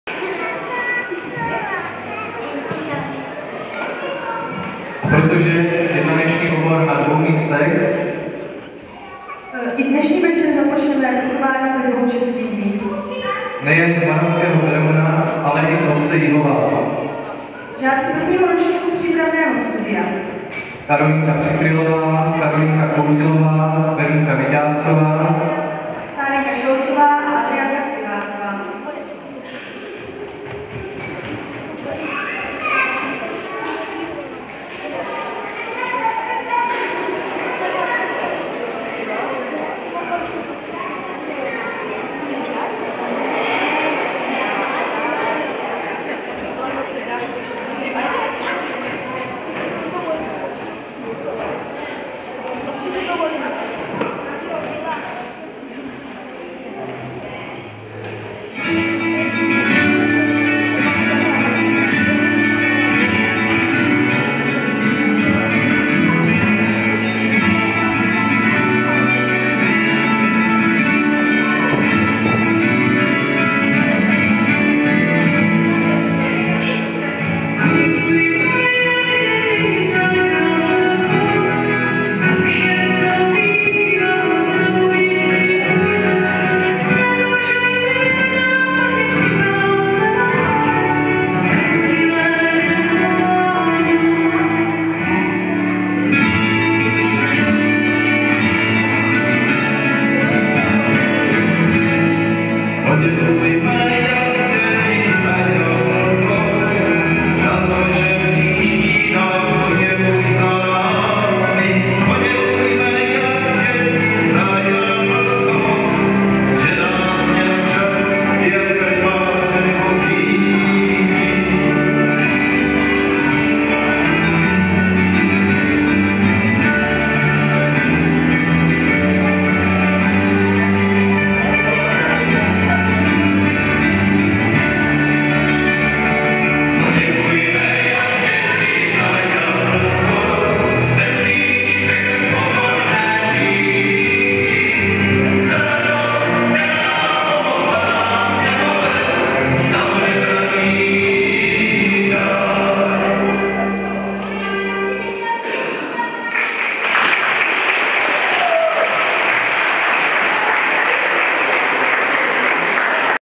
Cesta kolem světa 2010 - 2. ČR (Hradišťan)
2. české místo na počátku cesty. ZUŠ Moravský Beroun - pobočka Jívová. Choreografie: Tomáš Beníček